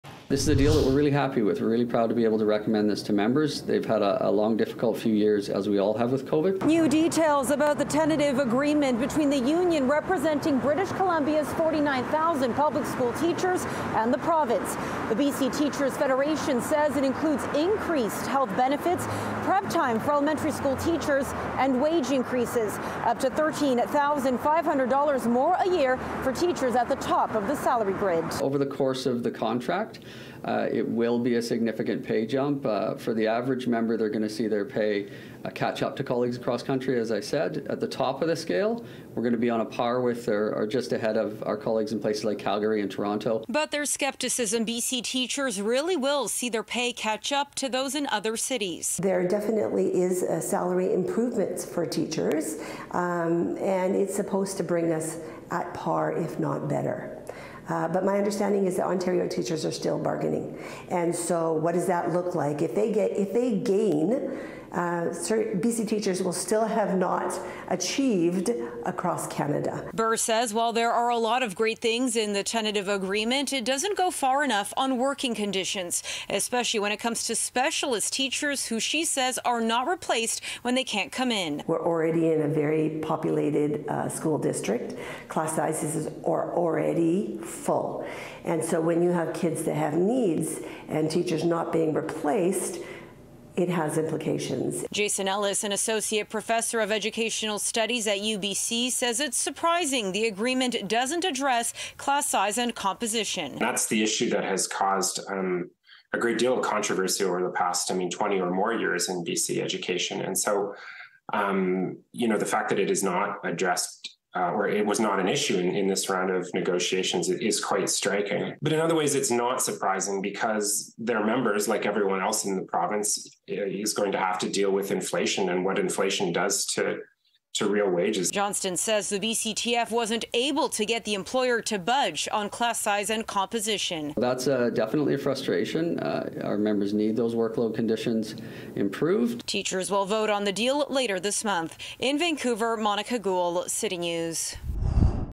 BCTF says deal with province will see ‘significant pay jump’ for teachers
The BCTF says the tentative agreement it reached with the province will see wages for B.C. teachers catch up to their colleagues across the country. Monika Gul reports the Surrey Teachers’ Association isn’t so sure.